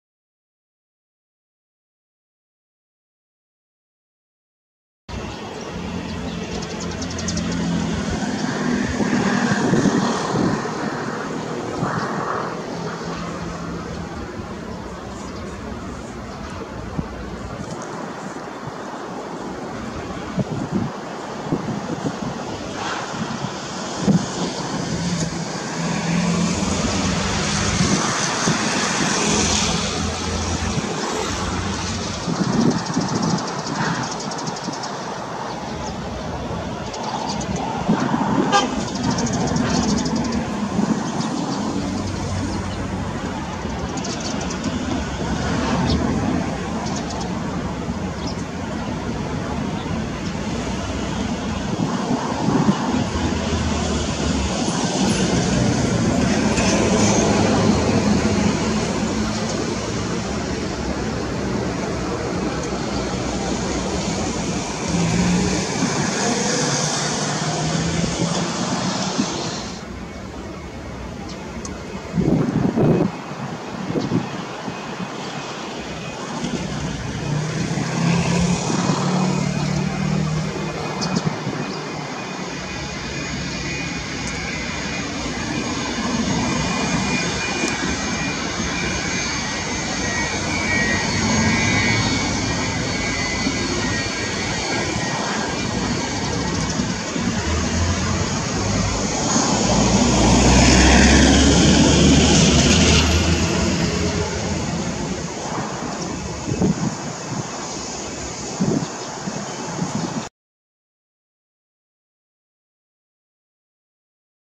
FAT MD-83 B-28007@TSA
RW28 landing Taipei Songshan International Airport